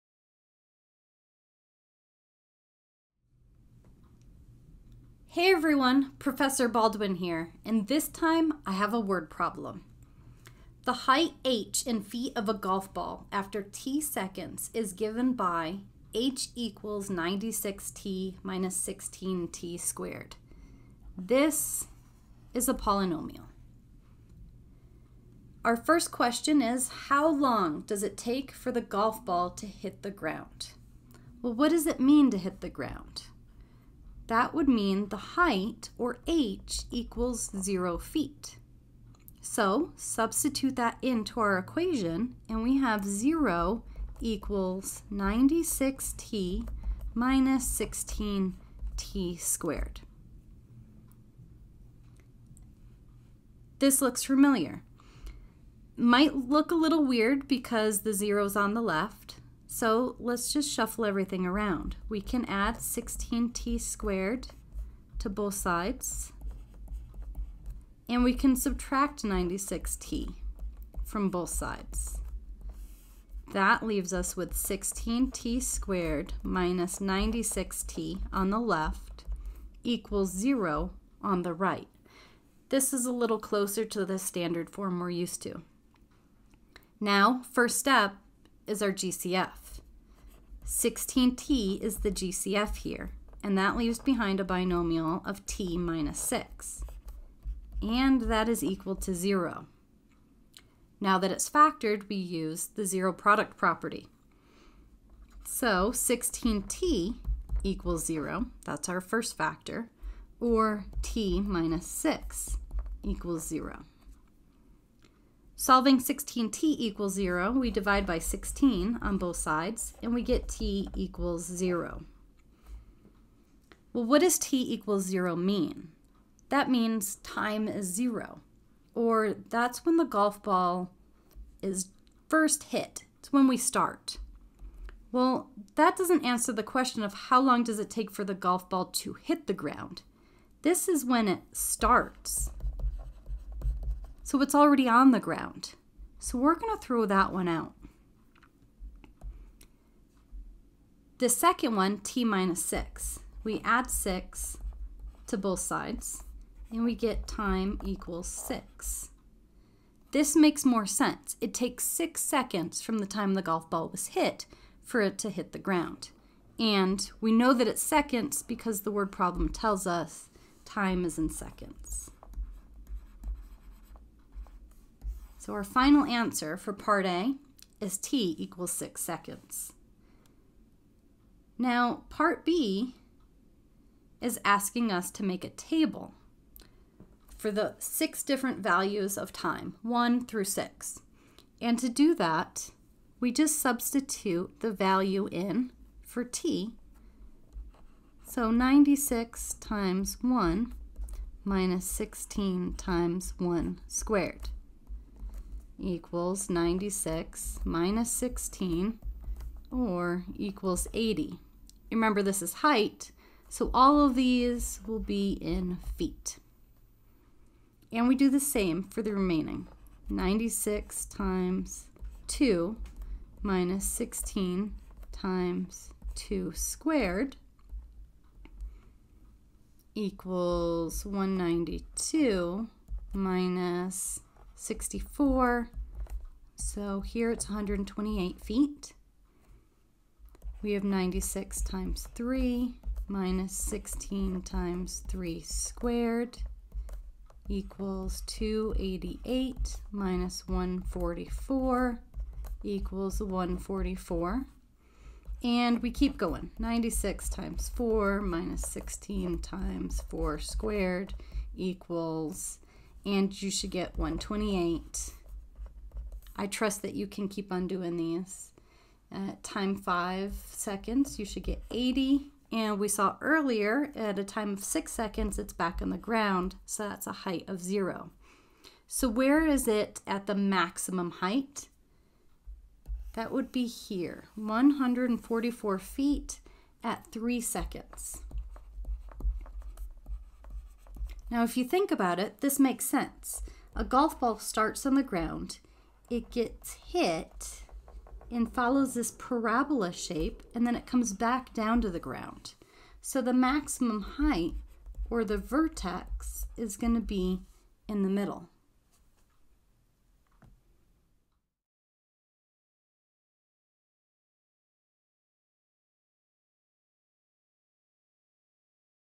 How long does it take for the golf ball to hit the ground? When does it reach maximum height? 6.6.67
Problem 6.6.67

The height h in feet of a golf ball after t seconds is given by h = 96t - 16t^2.

(a) How long does it take for the golf ball to hit the ground?

(b) Make a table of h for t=1,2,3,…6. After how many seconds does the golf ball reach its maximum height?